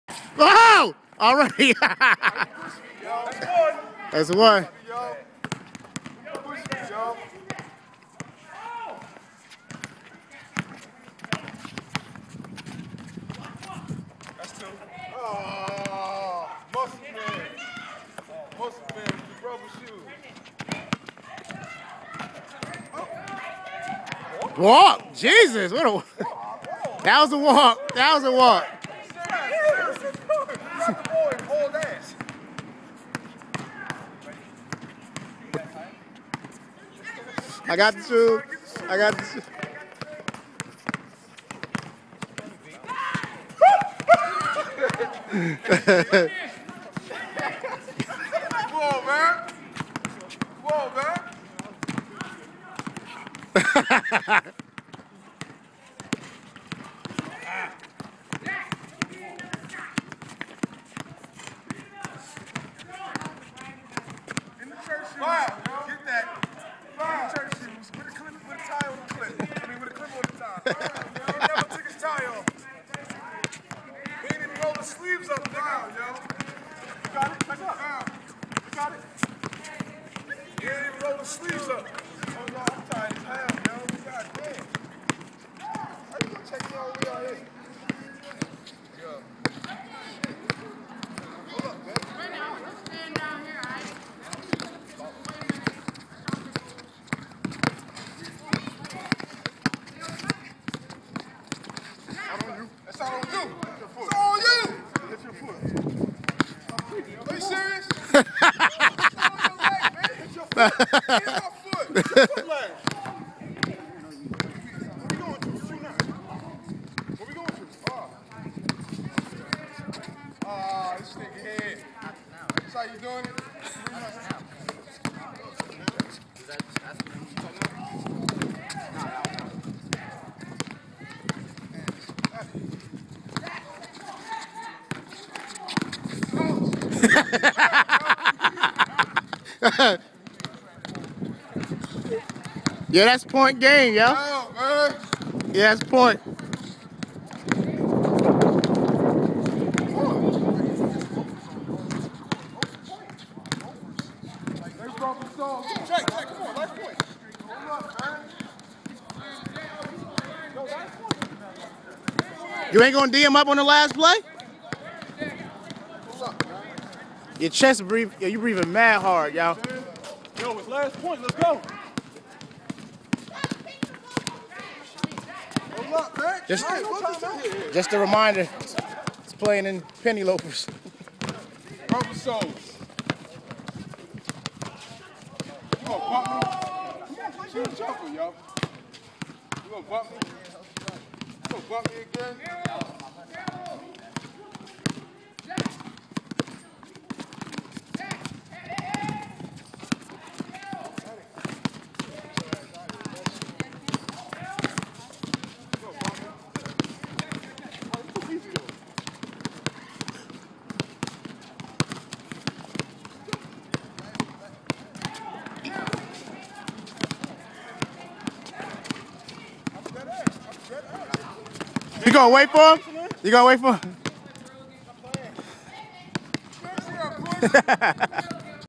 Garan beats Travis 5-0 wearing penny loafers!!
N*gga Travis ain't even score. All out of breath and sh*t. Cracked his ankle on the first play